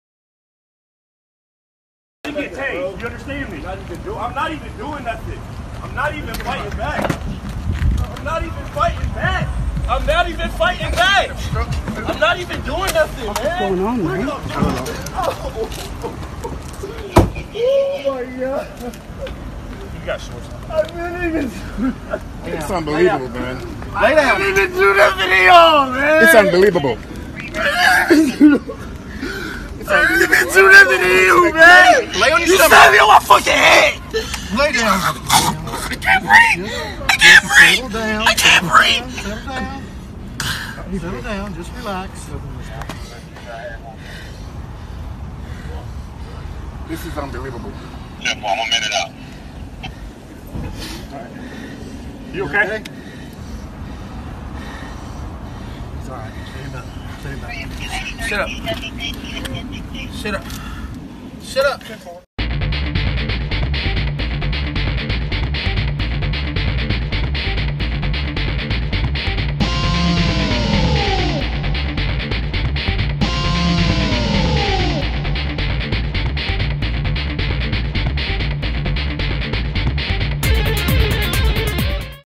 Cops Chock-Slam Ex-Footballer Desmond Marrow After Mistaking Phone For Gun | Lehren News
A police officer in the Atlanta suburbs has been fired after an internal investigation found he used unnecessary force in the arrest of a black man for possessing a firearm - which in reality, the man said, was his cellphone.